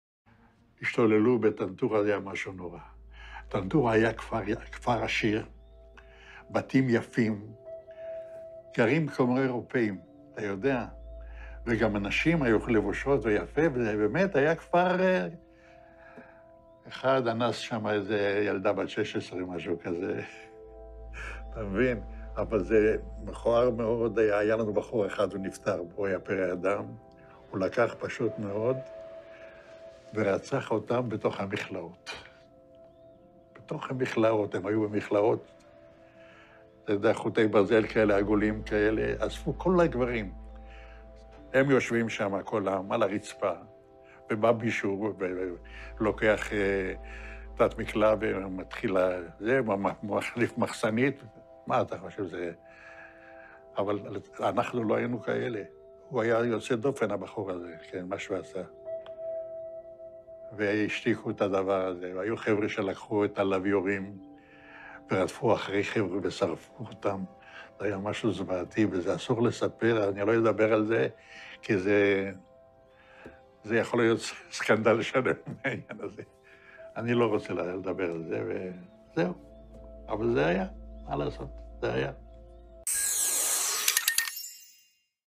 Former Israeli soldiers reveal new evidence of the Tantura massacre 1948
Former Israeli soldiers reveal new evidence of the Tantura massacre. Over 90 years old, several former soldiers of the Alexandroni brigade admitted that they committed a massacre in Tantura and described some of its scenes, which infers that the death toll is much higher to the 20 confirmed by the Army at the time.